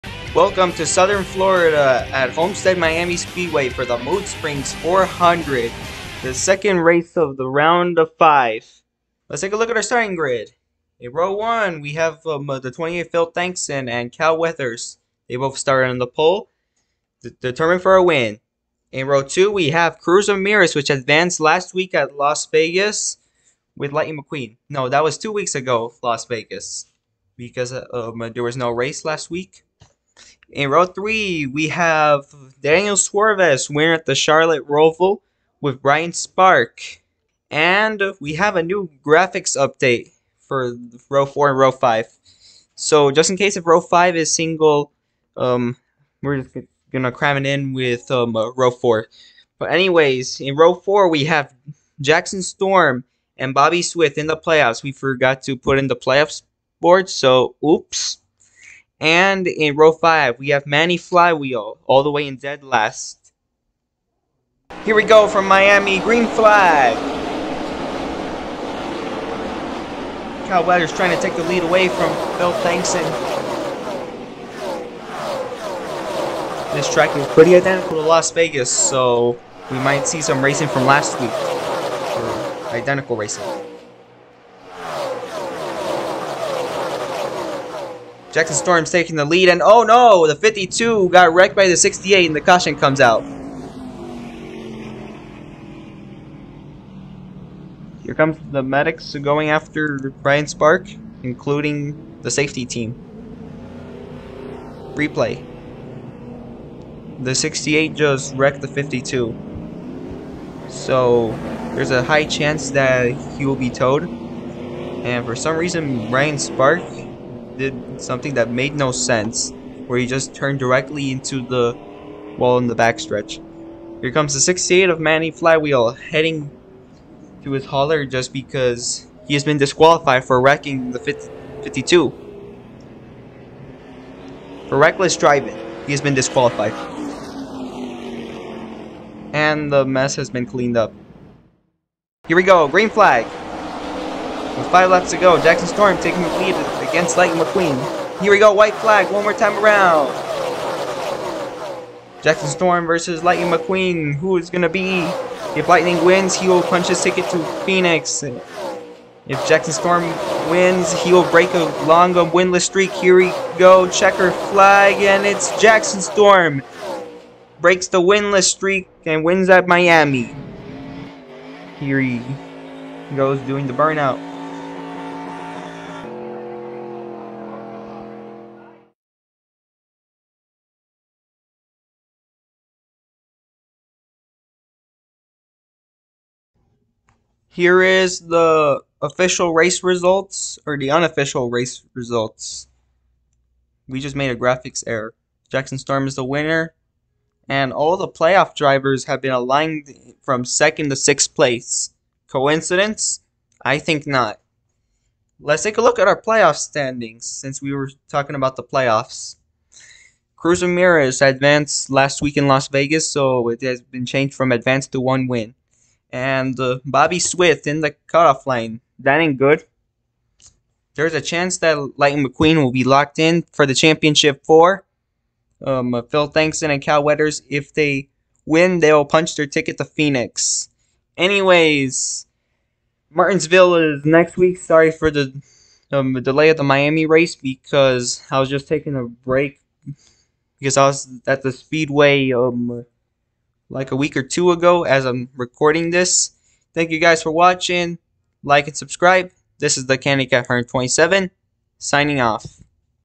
2023 Piston Cup Race 9 Mood Springs 400 at Homestead Miami Speedway
Welcome to Homestead-Miami Speedway for the 2nd race of the Round of 5 for the playoffs to see who will be in or out and who is worthy of a championship win.